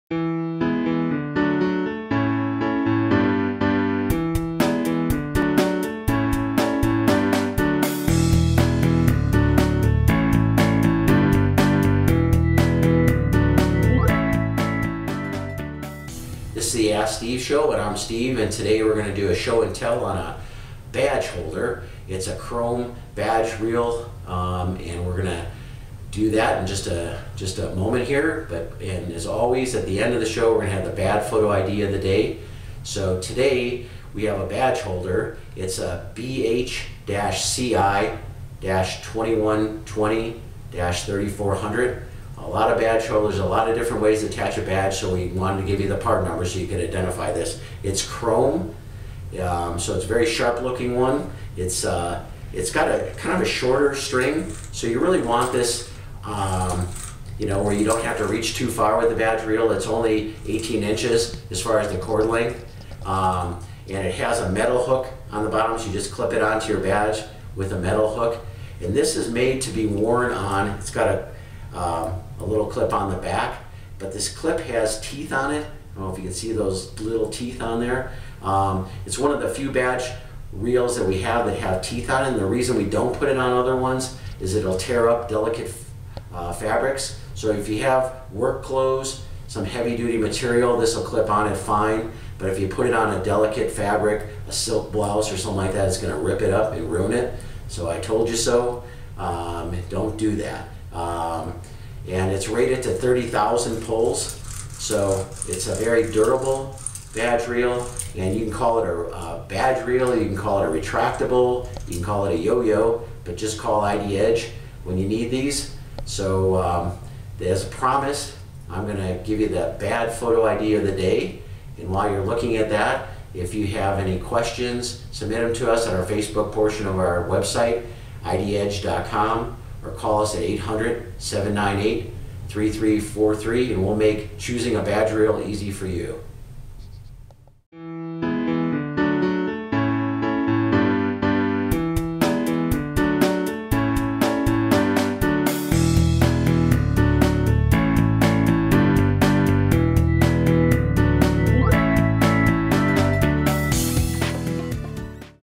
Chrome Badge Reel | Security Cards
Steve will be reviewing a Chrome Badge Reel in this short review.